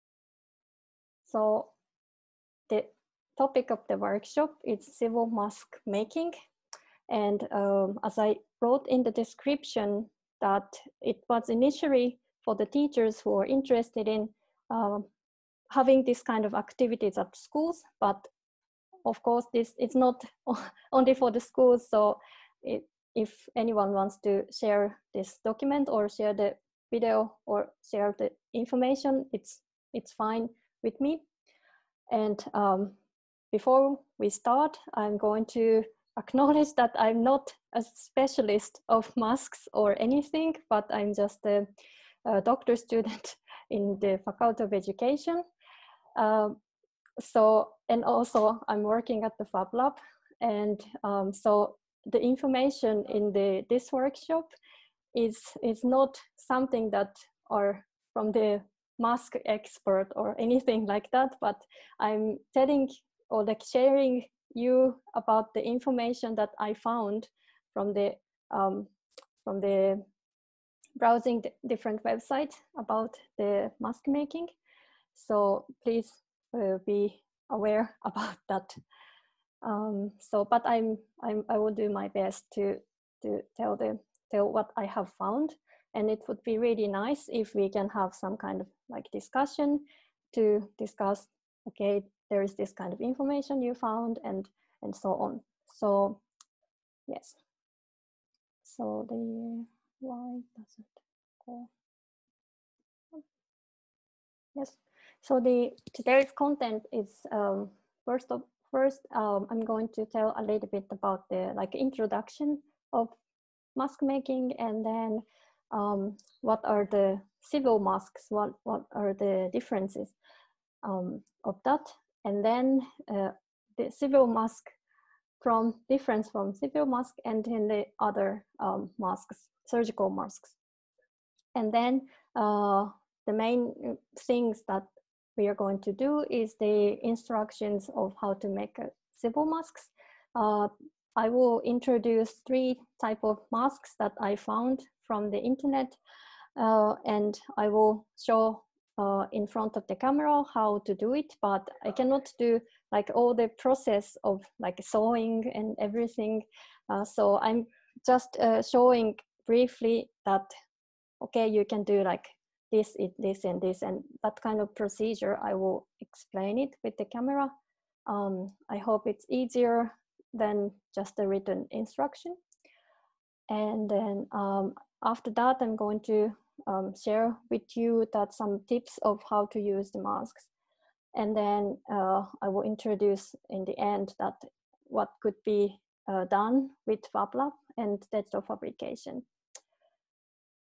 Civil Mask Making Workshop (I) Intro
Wearing masks is recommended to protect other people in closed places (e.g., public transport)  or places that is difficult to keep safety distance. This workshop introduces how to make DIY face masks with fabric (civil masks). Following the written instructions, instructor will explain the process of making three different types of civil masks. The tips of making and wearing masks will be also provided. The workshop will be online via zoom (2 hours). The workshop is primarily for teachers who want to implement activities at the school, but others are also welcome! On request, additional workshops for making stickers to personalise your civil masks using Inkscape (2D vector graphic software) and vinyl cutter will be arranged.